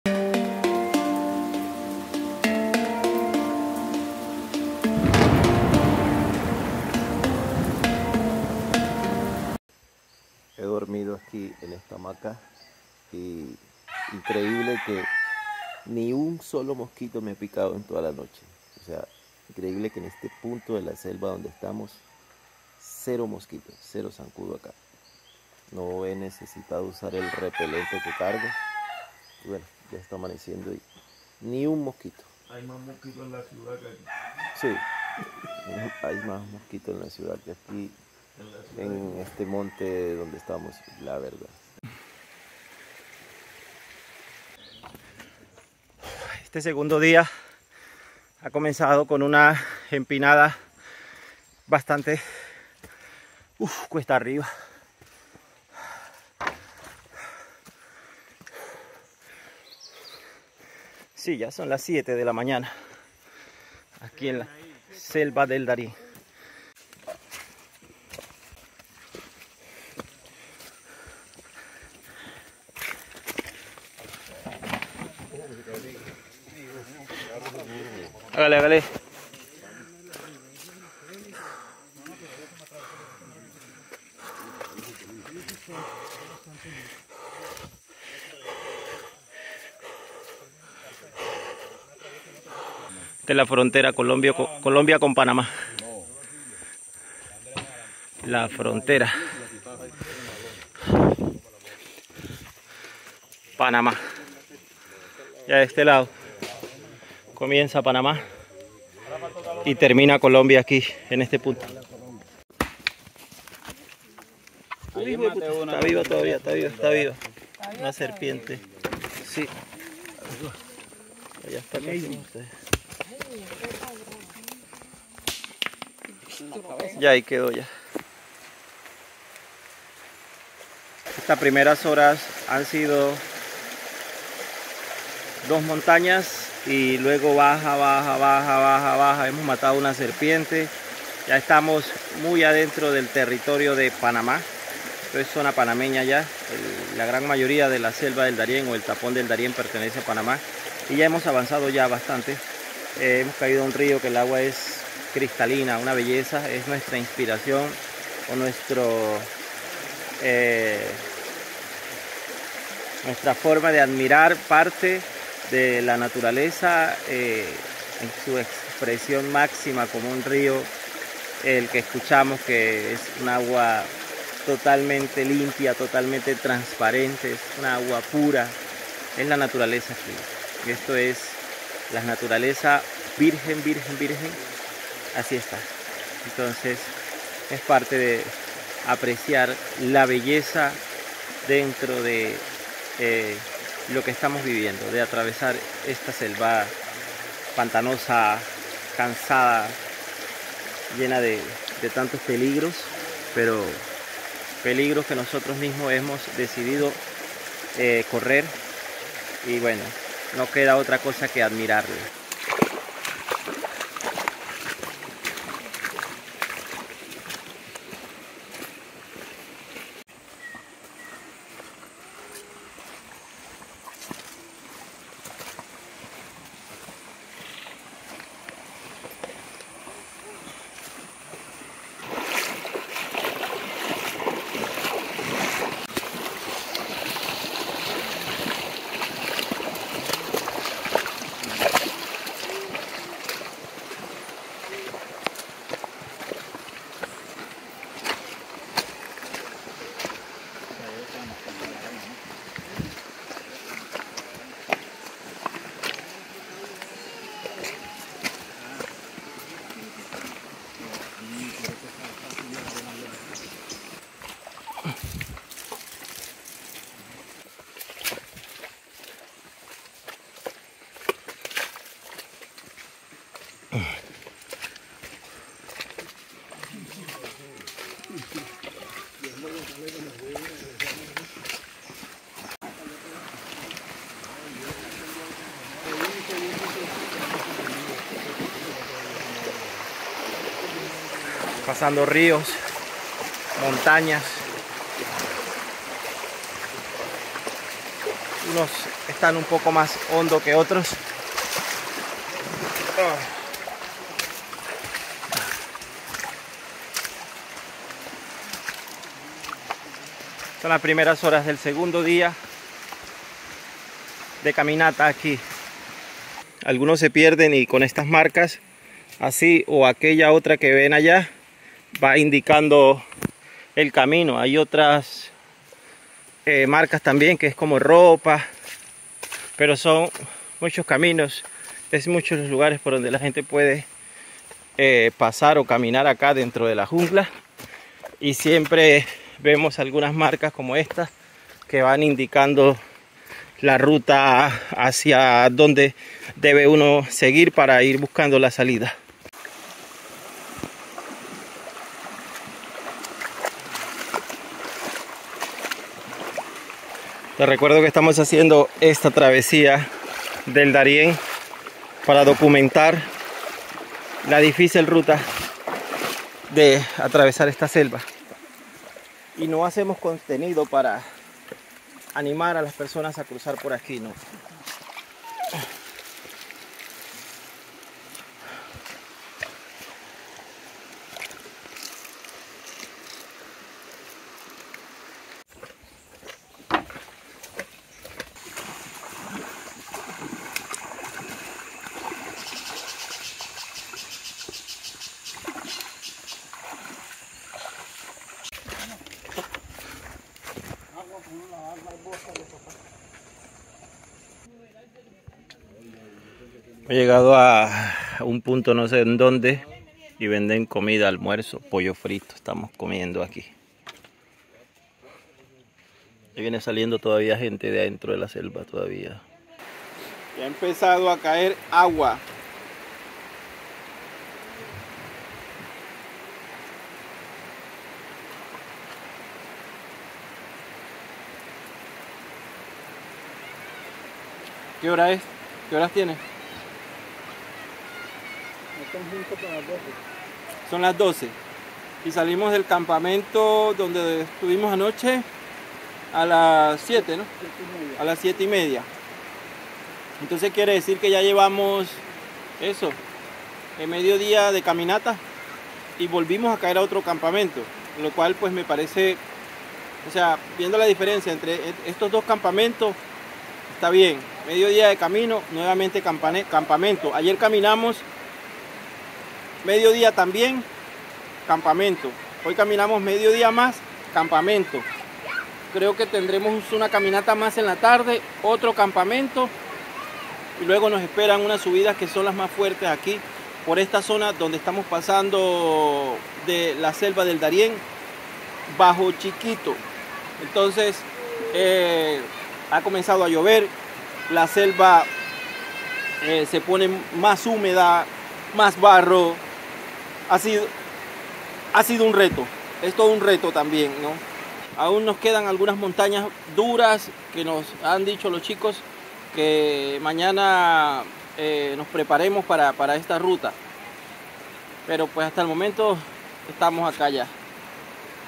Animales Y Peligros, DARIÉN - Día 6
Nos ha alcanzado la noche en las primeras aldeas del lado colombiano, y junto a los inmigrantes que están sorteando estos territorios buscando cruzar el Darién rumbo a Estados Unidos nos disponemos a pasar la noche en un campamento improvisado.

¡SÍGUEME!